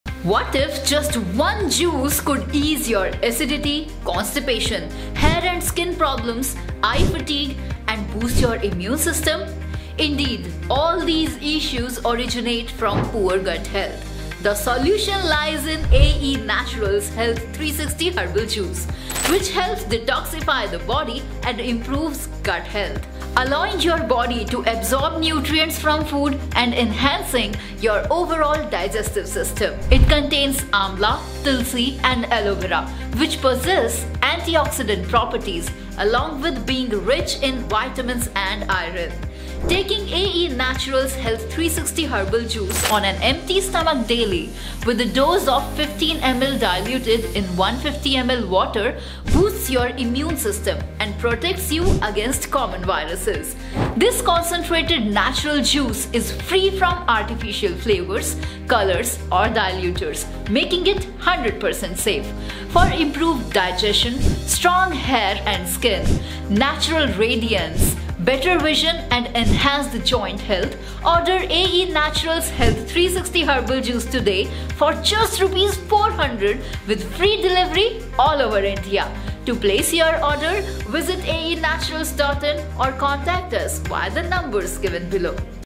How to Boost Immune System and Improve Gut Health | Health 360 Herbal Juice | English Version
How to Boost Immune System and Improve Gut Health | Health 360 Herbal Juice | Overall Health boost

How can one simple juice tackle gut, skin, and hair concerns all at once?

Yes, it is possible with Health 360 Herbal Juice! 🌿

Detoxify your body, rejuvenate your skin, and revitalize your hair with our organic blend of Ayurvedic herbs. Say goodbye to gut issues, hello to glowing health!

👉 One Solution, Multiple Benefits! 👈

💫 Improve gut health
💫 Boost skin radiance
💫 Enhance hair strength

Ready to experience 360-degree wellness?